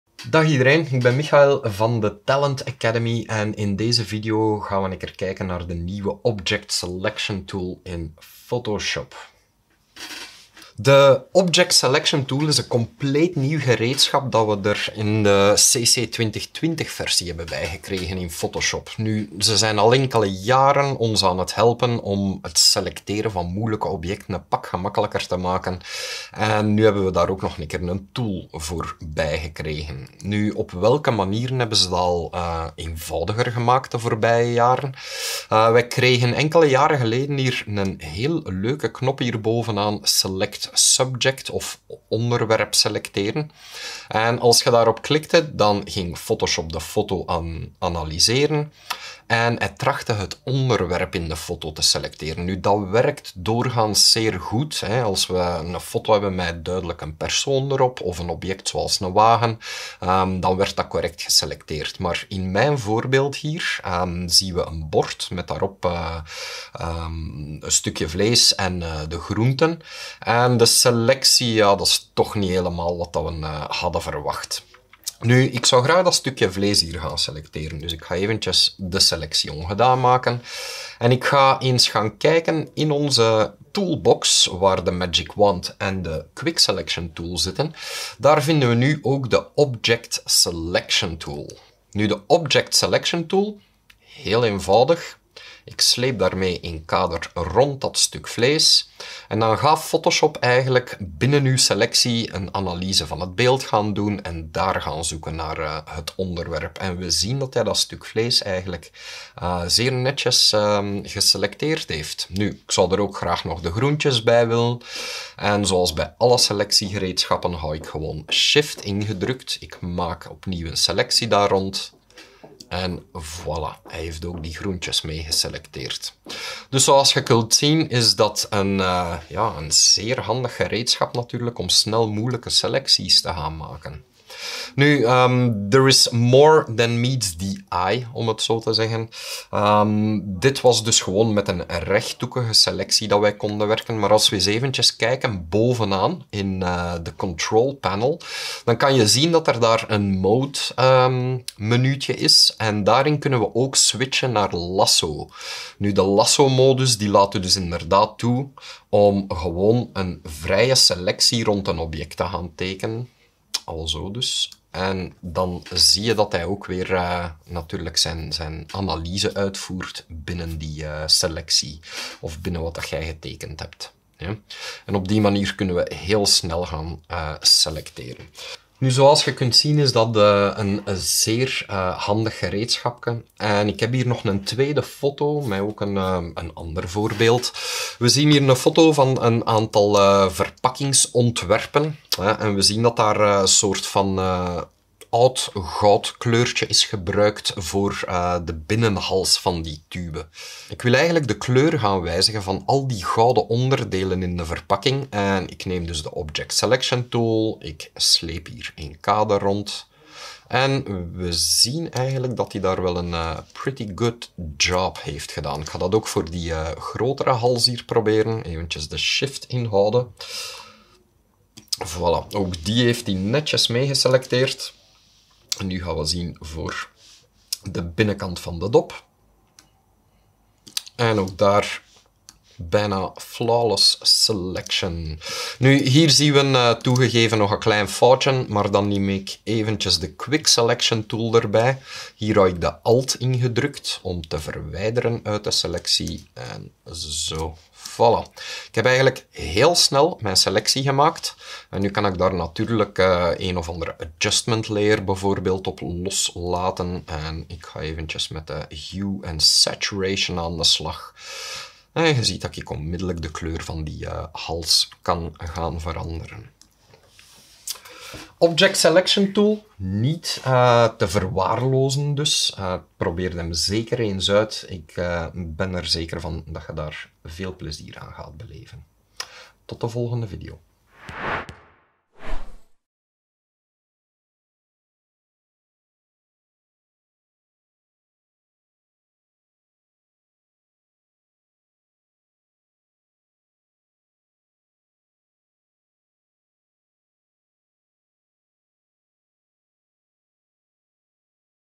Object Selection Tool in Photoshop 2020
De nieuwe Object Selection Tool in Photoshop 2020!
De kracht van Select subject gecombineerd met een selectiegereedschap. Ontdek hoe deze nieuwe tool jouw leven makkelijker kan maken.